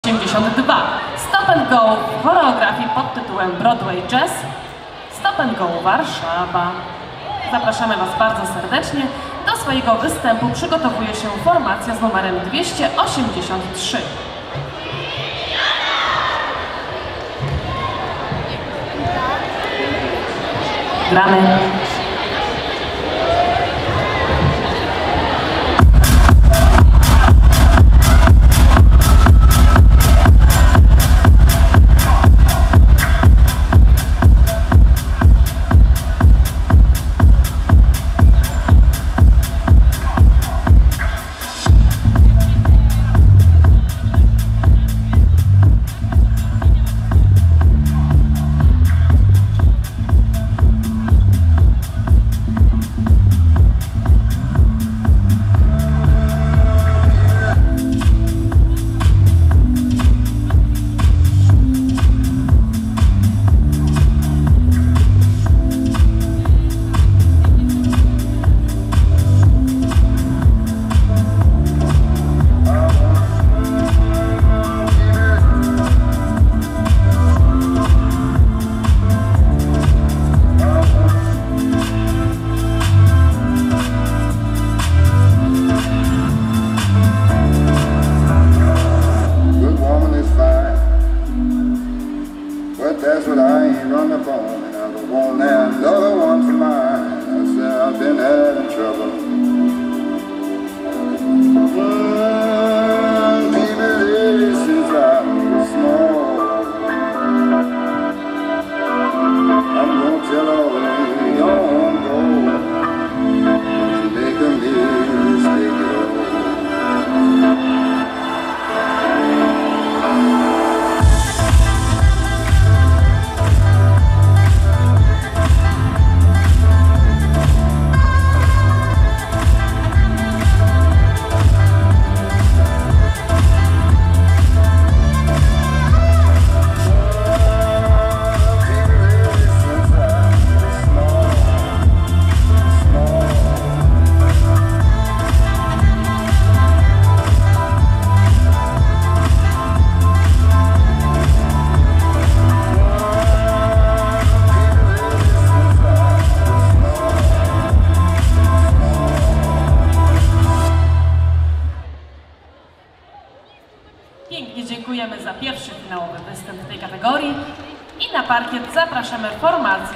TfD 2021 "Stop And Go - Broadway Jazz" 4M JAZZ FORMACJE DOROŚLI
Time for Dance 2021 - Legionowo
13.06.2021

Jazz Formacje Dorośli

Stop And Go - Broadway Jazz 4 miejsce

Stop And Go Warszawa